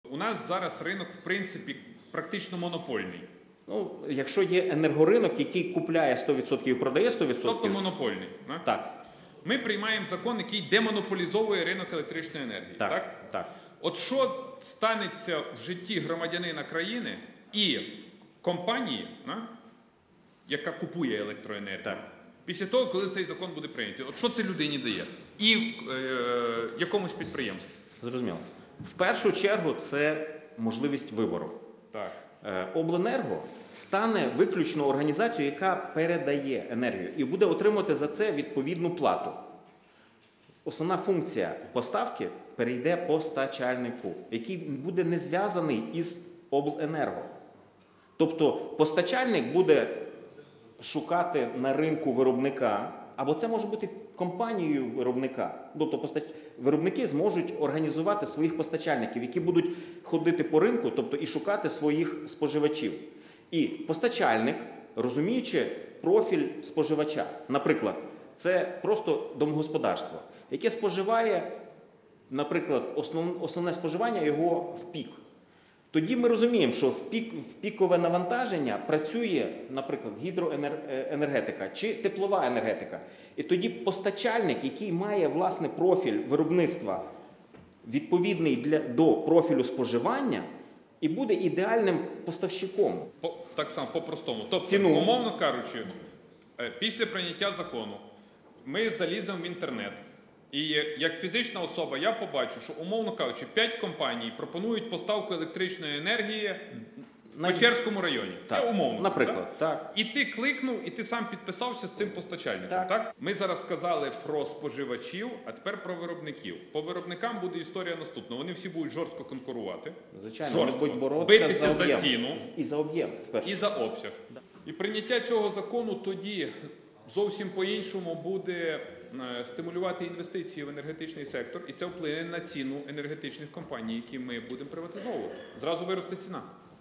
Уряд направить до Парламенту законопроект, який демонополізує ринок електричної енергії
Уряд України схвалює та направляє до Парламенту проект закону про ринок електроенергії, який дозволяє демонополізувати ринок, дає змогу підприємствам заключати прямі договори з виробниками електроенергії, а громадянам в подальшому - самостійно обирати постачальника. Про це на засіданні Кабінету Міністрів у середу, 2 березня, повідомив Прем’єр-міністр України, лідер «Народного фронту» Арсеній Яценюк.

Проект закону про ринок електроенергії, який запроваджує зрозумілі, прозорі та чесні механізми ринкових відносин в електроенергетичній галузі, представив на засіданні Уряду Міністр енергетики та вугільної промисловості України Володимир Демчишин.

Арсеній Яценюк підкреслив, що ухвалення цього проекту дозволить демонополізувати ринок електричної енергії.

Згідно з документом, громадяни України зможуть самостійно обирати постачальника електроенергії. Таким чином, зазначив Арсеній Яценюк, буде змога обрати постачальника з найбільш привабливим тарифним планом. Великі підприємства, у свою чергу,  отримають можливість заключати прямі договори з виробниками.

Глава Уряду підкреслив, що законопроект також  дасть змогу створити конкуренцію серед виробників електроенергії:  «Вони всі будуть жорстко конкурувати за ціну і за обсяг (постачання)».

Ухвалення цього закону, наголосив він, дозволить підвищити інвестиційну привабливість української електроенергетики: «Це вплине і на ціну енергетичних компаній, які будуть приватизовуватися. Зразу виросте ціна».

Арсеній Яценюк зазначив, що цей проект закону в цілому схвалений західними партнерами.

Глава Уряду наголосив на важливості того, аби закон про ринок електроенергії повністю відповідав європейським нормам та виключав можливість ручного управління на ринку.

Прем’єр-міністр доручив Міністерству  енергетики та вугільної промисловості разом із Секретаріатом Кабінету Міністрів доопрацювати законопроект за 24 години : «Закон схвалюємо, відправляємо в Парламент, і очікуємо на всеосяжну підтримку українським Парламентом процесу реформ».

Приєднуйся!